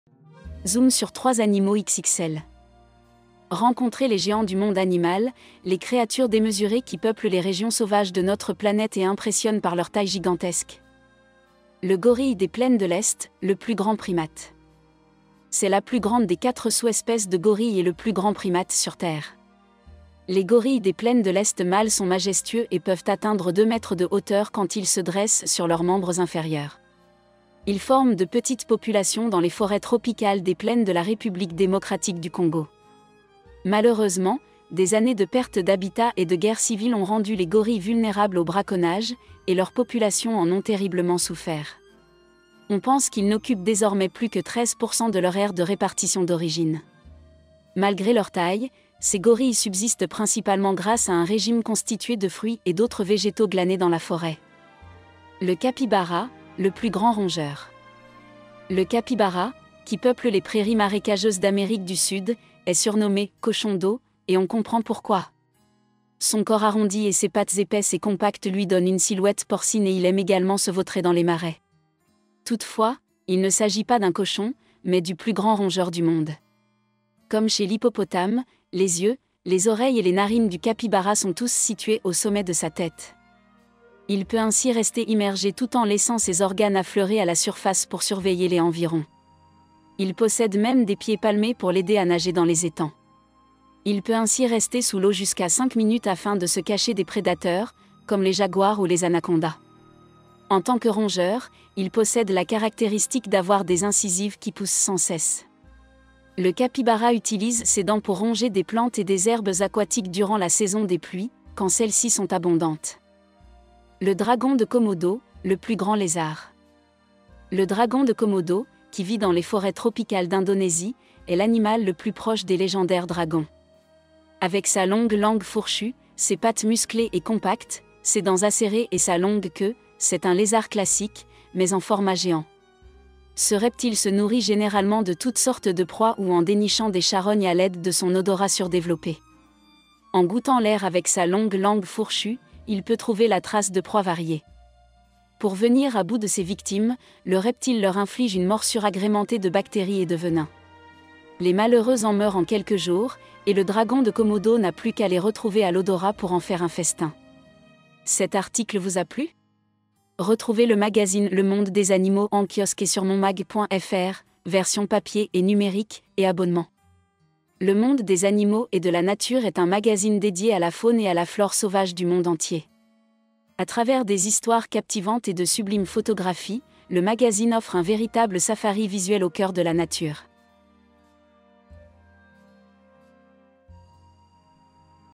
Zoom sur 3 animaux XXL
Rencontrez les géants du monde animal, les créatures démesurées qui peuplent les régions sauvages de notre planète et impressionnent par leur taille gigantesque !. From 20minutes
MAKE SURE TO FOLLOW ON MY VIDEOS AT SANPISETH.NET